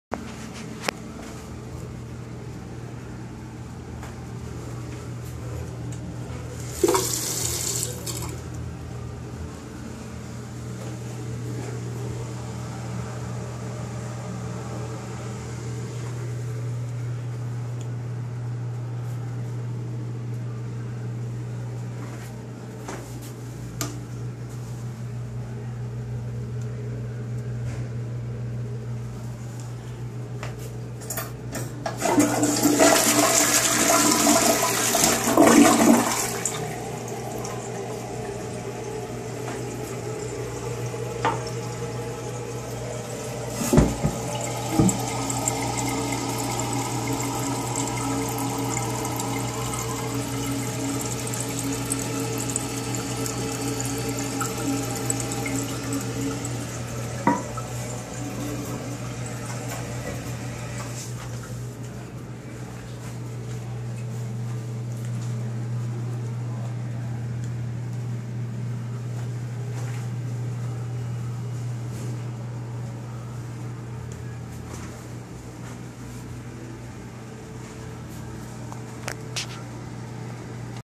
194: A Pita Pit bathroom, featuring a Toto toilet.
This bathroom could use a good cleaning.